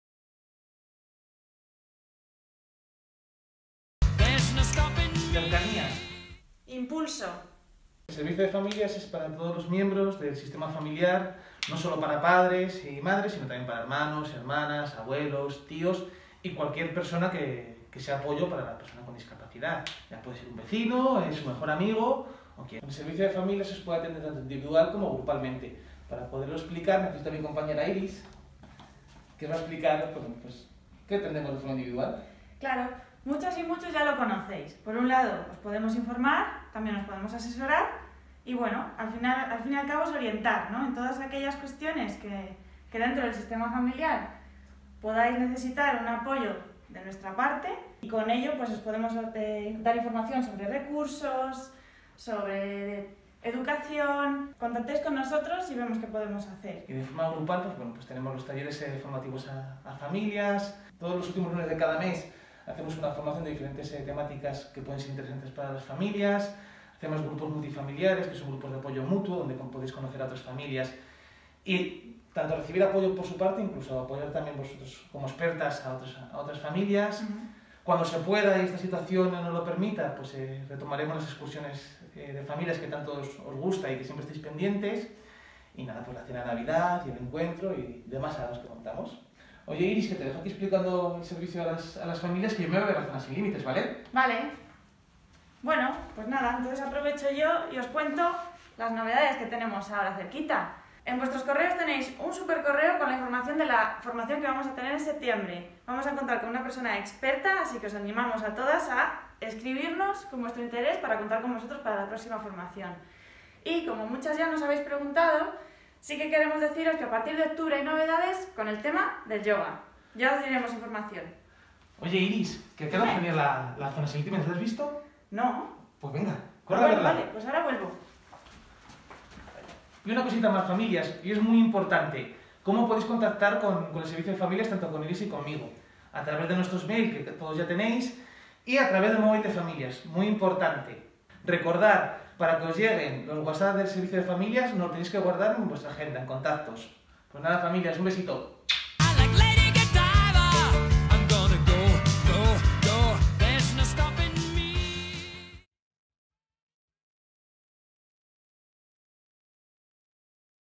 Presentación Servicio de Familias Adisli
Presentación Curso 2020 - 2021 con Oscar Meléndez e Iris de la Fuente. ¡Te esperamos!